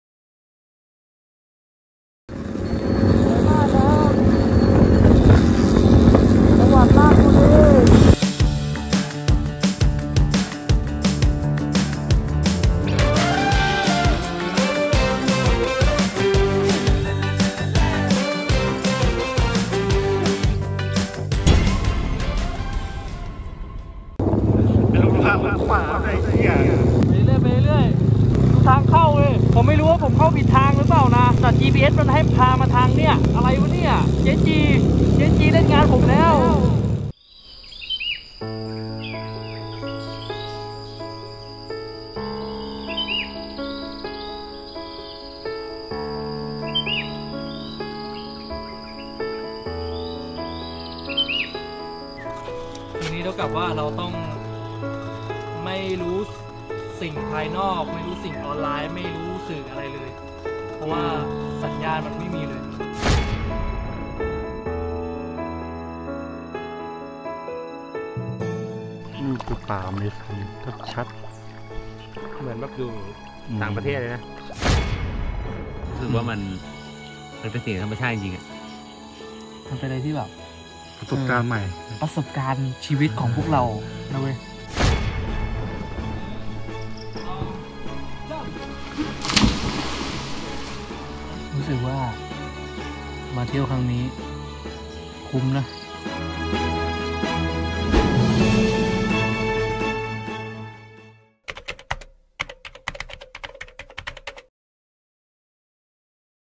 [กางเต็นท์ไปกับเรา] Trailer ไร่ยุทธนา สวนผึ้งราชบุรี
การเดินทางครั้งแรกของพวกเรา ที่ได้ไปใช้ชีวิตท่ามกลางหุบเขาแบบไร้สัญญาณมือถือ กางเต้นท์ตั้งแคมป์ในรูปแบบของเรา ได้ไปอยู่กับธรรมชาติ บรรยากาศ ทิวทัศน์ ที่แตกต่างกันไป และเป็นประสบการณ์ใหม่ในชีวิต